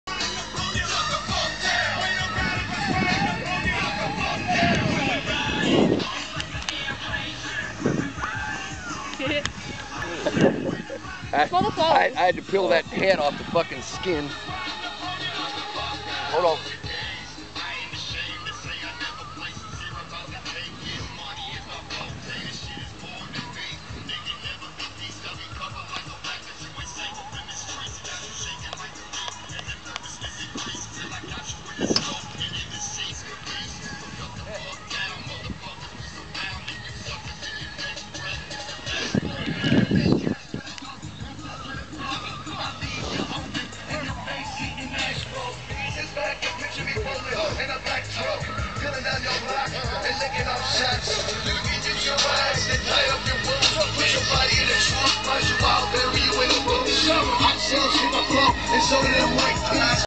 DISTANCE CLARITY!
CHECKING THE CLARITY VS. DISTANCE IN THIS VIDEO, WITH NO POWER TO THE SUBS!

PLENTY OF VIDEOS TO VIEW THAT HAVE ALREADY BEEN UPLOADED!